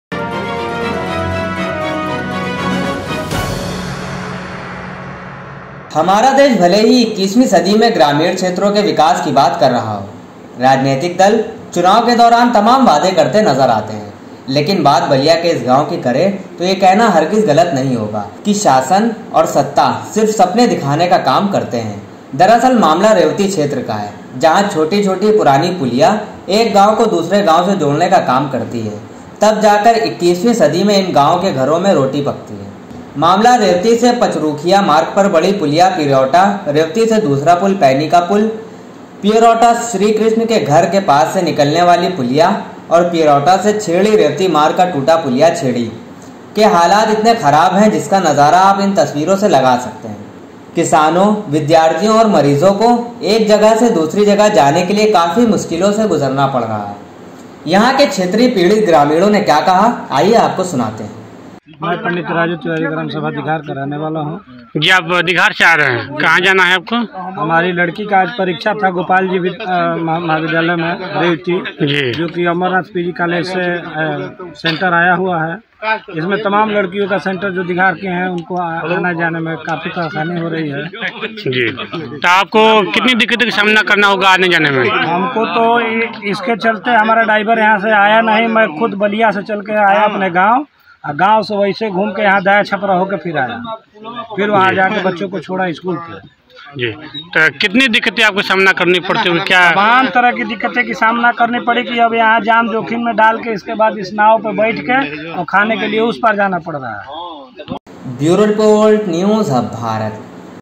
21वीं सदी के भारत के गाँव की ऐसी तस्वीरें देखकर हो जाएंगे हैरान, सरकार के दावे फेल | News Hub Bharat
About this video-उत्तर प्रदेश बलिया जिले में हमारा देश भले ही 2021 वीं सदी में ग्रामीण क्षेत्रो के विकास की बात कर रहा हो। राजनैतिक दले चुनाव के दौरान तमाम वादे करते नजर आते है। लेकिन बात बलिया के इस गांव की करे तो ये कहना हरगिज़ गलत नही होगा कि शासन-सत्ता सिर्फ सपने दिखाने का काम करते है। दरअसल मामला रेवती क्षेत्र का है जहाँ छोटी-छोटी पुरानी पुलिया एक गांव को दूसरे गांव से जोड़ने का काम करती है तब जा कर 21वीं सदी में इन गांवों के घरों में रोटी पकती है।
मामला रेवती से पचरुखिया मार्ग पर बड़ी पुलिया पियरौटा, रेवती से  दुसरा पुल पैनी का पुल,पियरौटा श्री कृष्ण सिंह के घर के पास से निकलने वाली पुलिया और पियरौटा से छेड़ी रेवती मार्ग का टुटा पुलिया छेड़ी। के हालात इतने खराब है जिसका नज़ारा आप इन तस्वीरों से लगा सकते है। ग्रामीणों, व्यापारियों,किसानों विद्यार्थियों और मरजो को एक जगह से दूसरे जगह जाने के लिए काफी मुश्किलों से गुजरना पड़ रहा है। सुने क्या कुछ कह यहां के क्षेत्रीय पीड़ित ग्रामीणों ने।